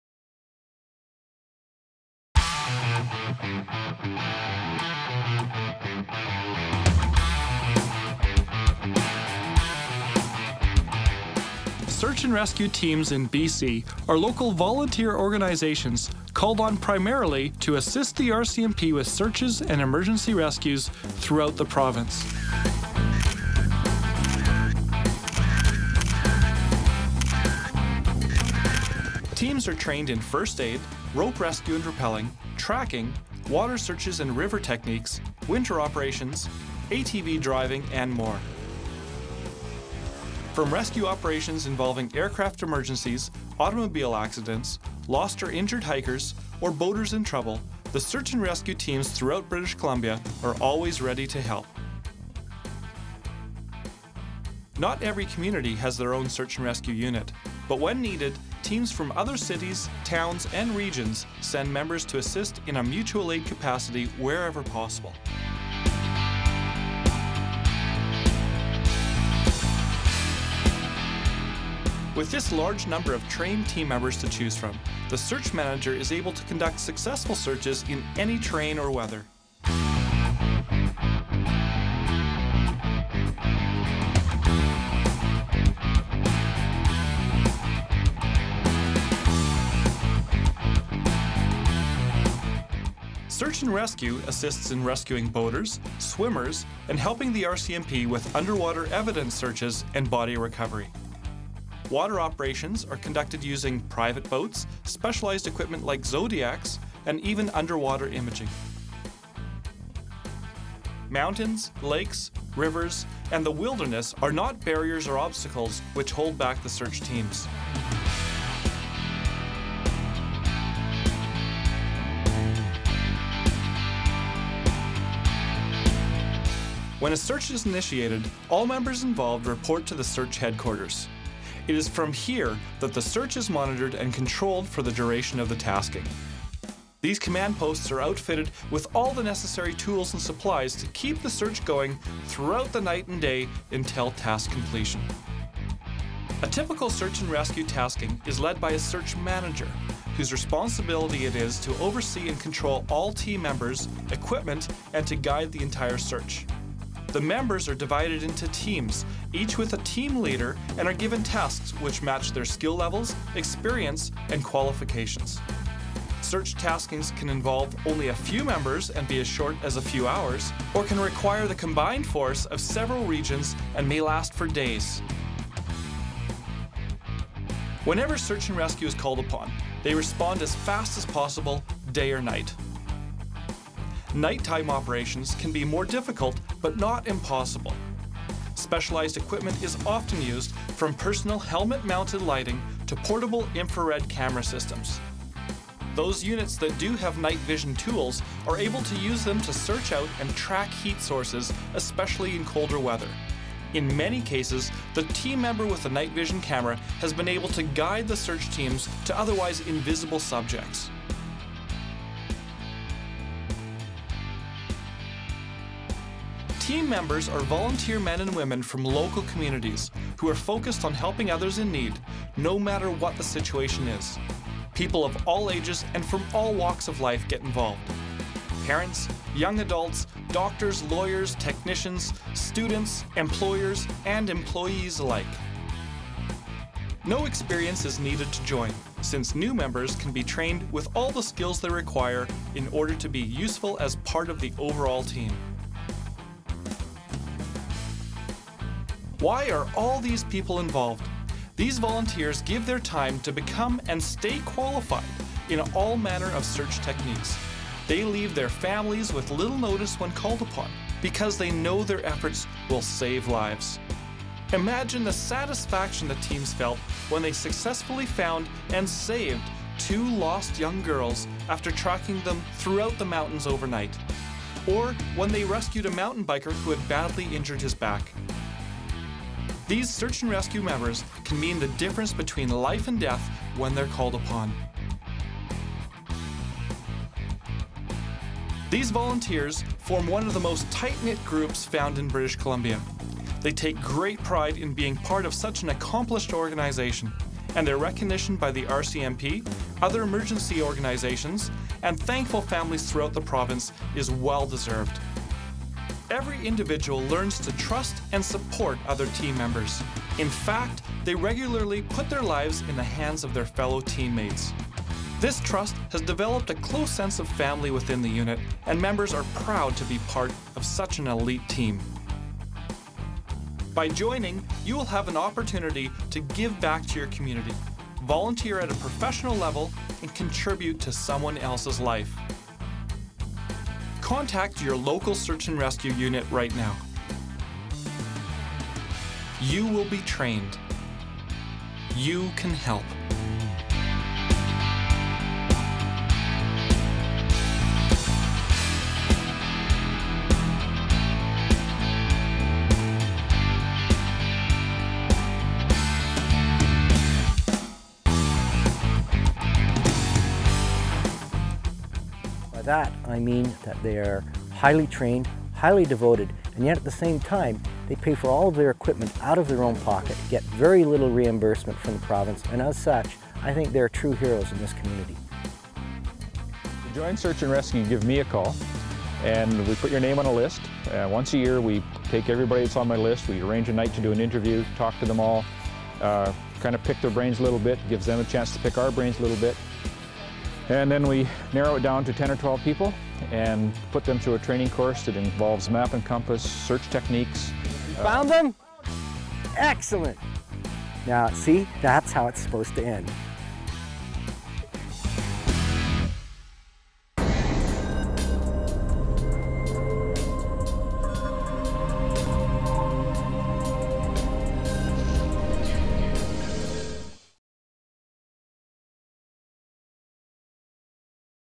An old BCSARA Promotion Video
This video was done in cooperation with a National SAR Secretariat NIF Grant a few years back under the leadership of Vernon Search and Rescue.